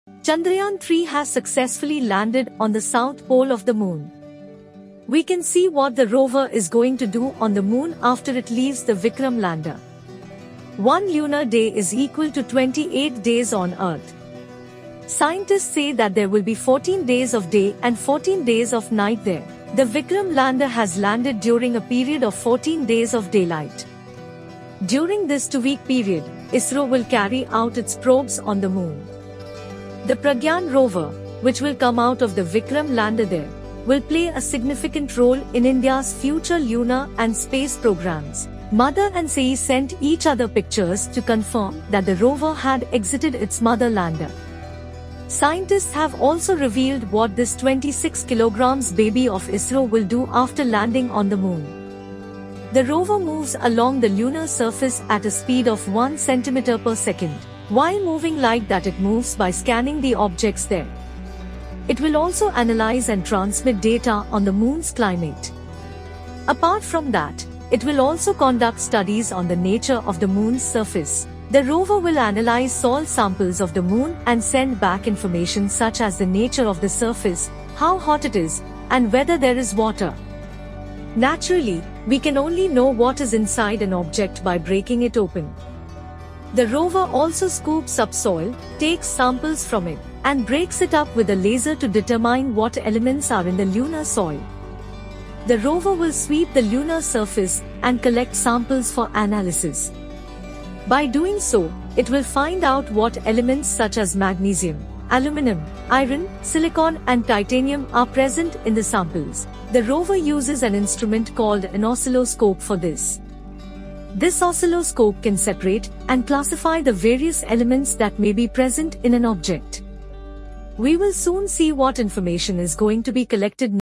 What this 26 kg baby of ISRO will do after landing on the moon | Worldwatch360 | News Plus
Chandrayaan 3 has successfully landed on the South Pole of the Moon. We can see what the rover is going to do on the moon after it leaves the Vikram lander.
One lunar day is equal to 28 days on Earth. Scientists say that there will be 14 days of day and 14 days of night there.
The Vikram lander has landed during a period of 14 days of daylight. During this two-week period, ISRO will carry out its probes on the moon.
The Pragnan rover, which will come out of the Vikram lander there, will play a significant role in India's future lunar and space programmes. Mother and Child sent each other pictures to confirm that the rover had exited its mother lander.
Scientists have also revealed what this 26 kg baby of ISRO will do after landing on the moon.
The rover moves along the lunar surface at a speed of one centimeter per second. While moving like that it moves by scanning the objects there.
It will also analyze and transmit data on the Moon's climate. Apart from that, it will also conduct studies on the nature of the Moon's surface. The rover will analyze soil samples of the moon and send back information such as the nature of the surface, how hot it is, and whether there is water.
Naturally, we can only know what is inside an object by breaking it open. The rover also scoops up soil, takes samples from it, and breaks it up with a laser to determine what elements are in the lunar soil.
The rover will sweep the lunar surface and collect samples for analysis. By doing so, it will find out what elements such as magnesium, aluminum, iron, silicon and titanium are present in the samples. The rover uses an instrument called an oscilloscope for this. This oscilloscope can separate and classify the various elements that may be present in an object. We will soon see what information is going to be collected next.